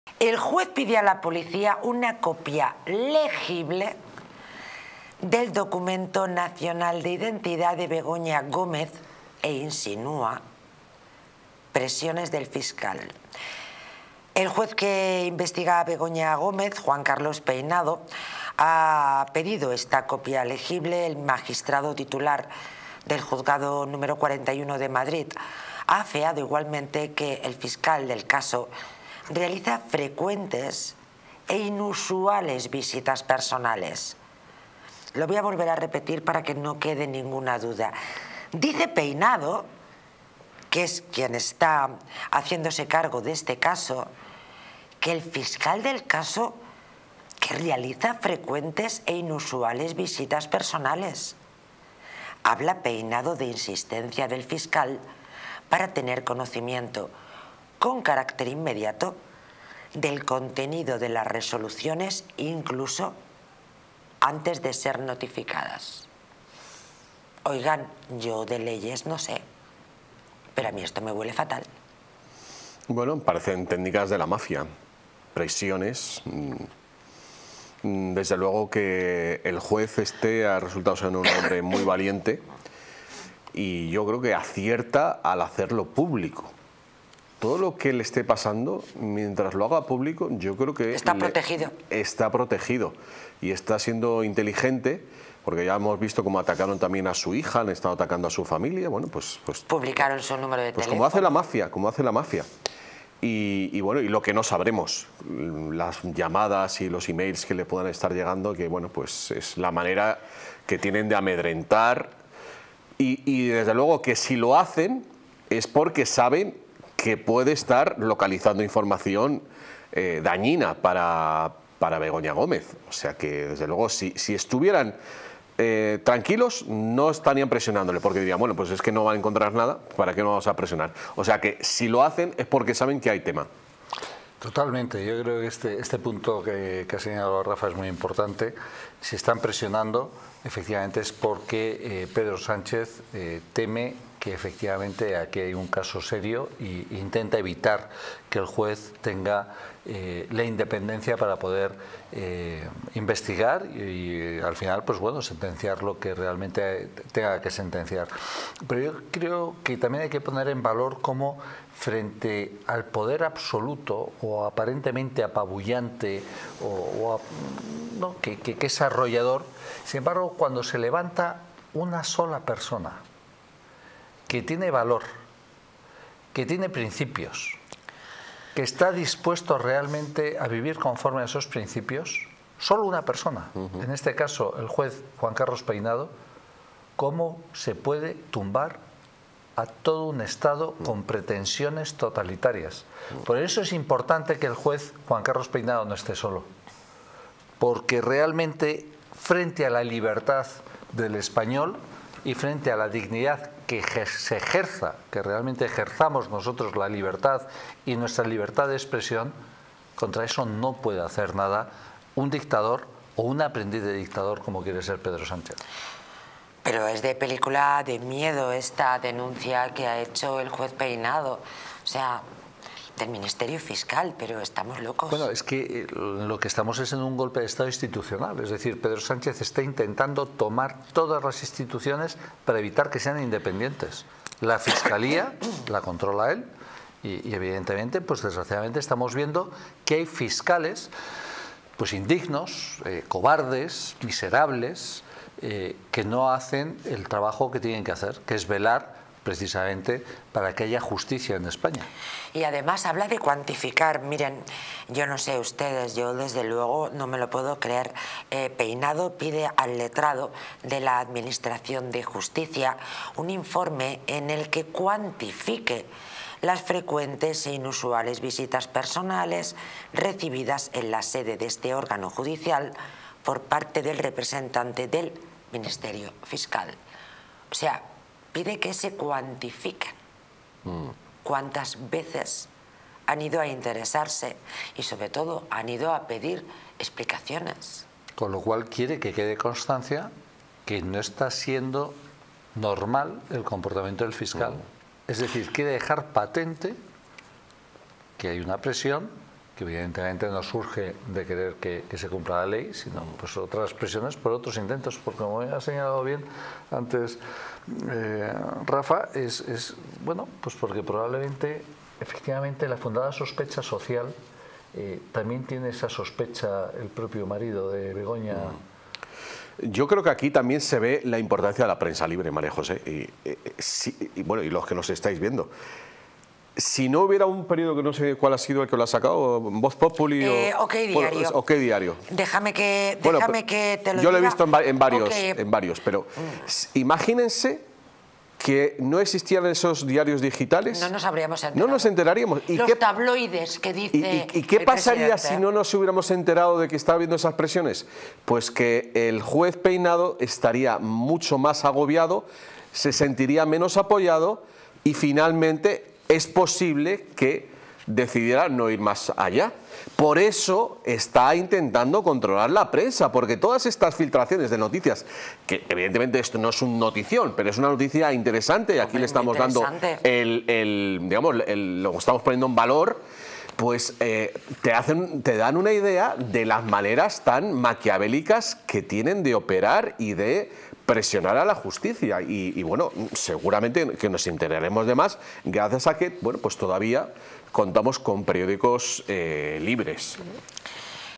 🔴El juez pide a la Policía una copia del DNI de Begoña Gómez "visto el estado de las actuaciones🔴
En este video, el juez pide a la Policía una copia del DNI de Begoña Gómez debido al estado de las actuaciones. Mantente informado con Distritotv.